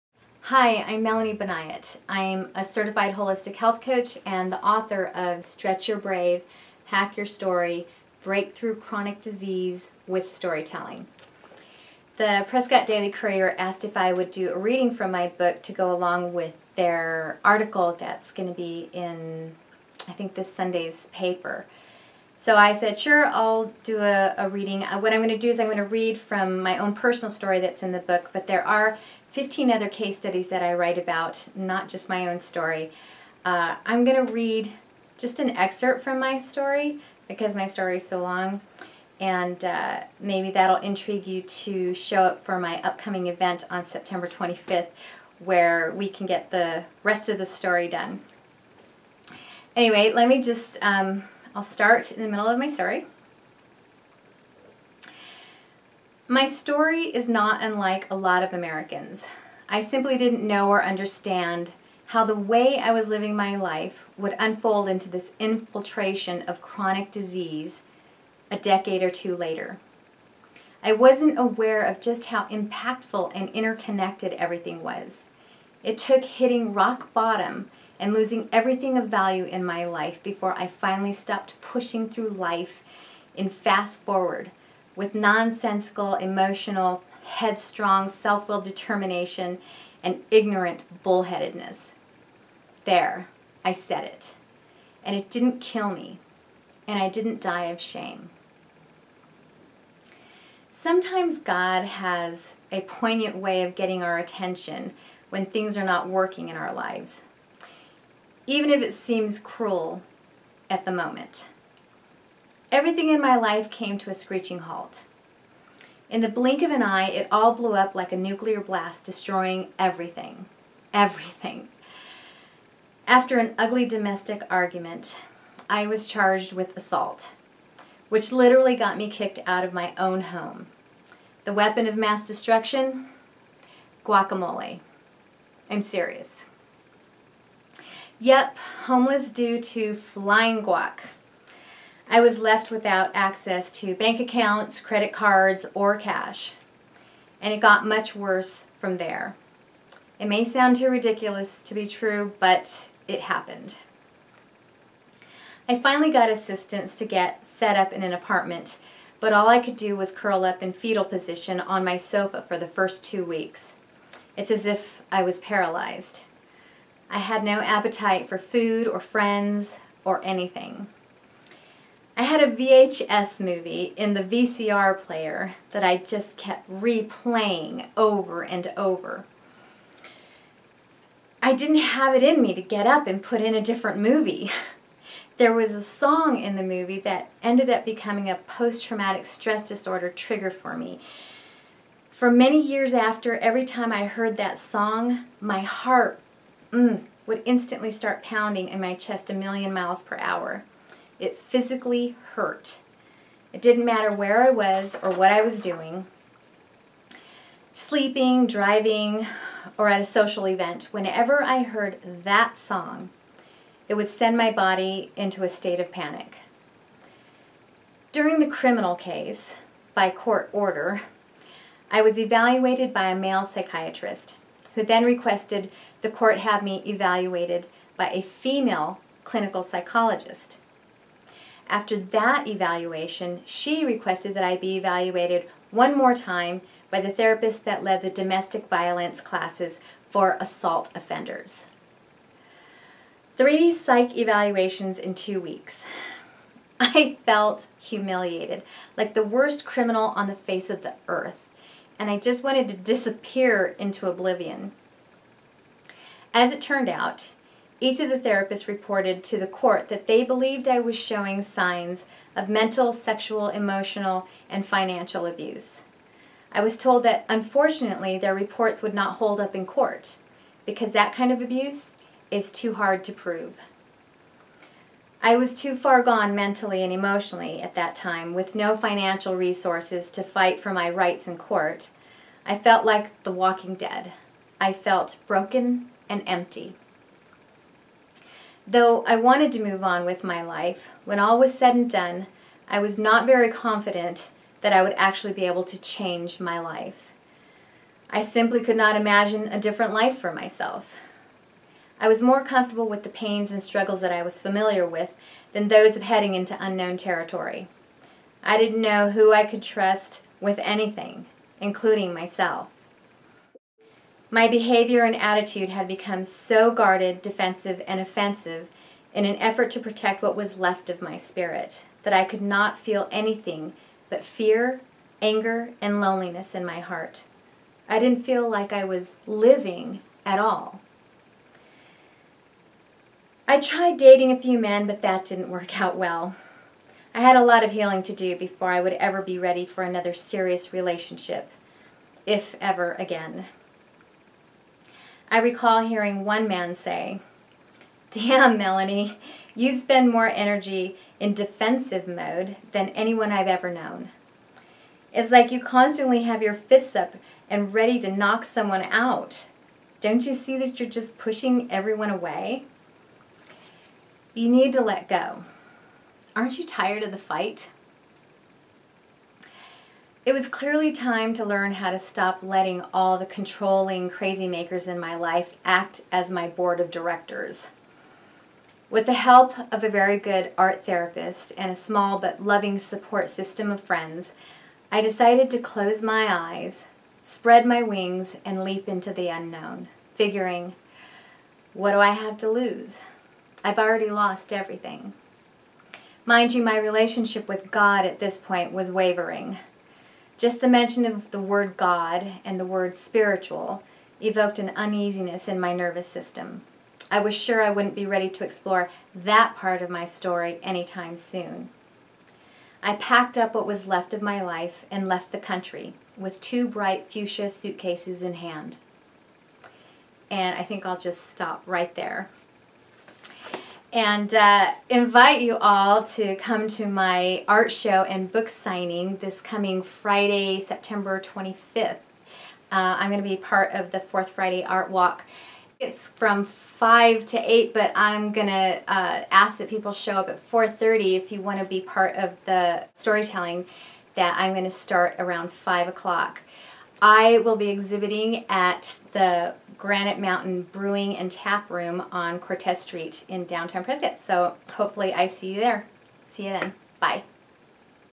'Stretch Your Brave' -- Book Reading by Melanie Banayat
Melanie Banayat reads an excerpt from her book, Stretch Your Brave, Hack Your Story: Break Through Chronic Disease with Storytelling.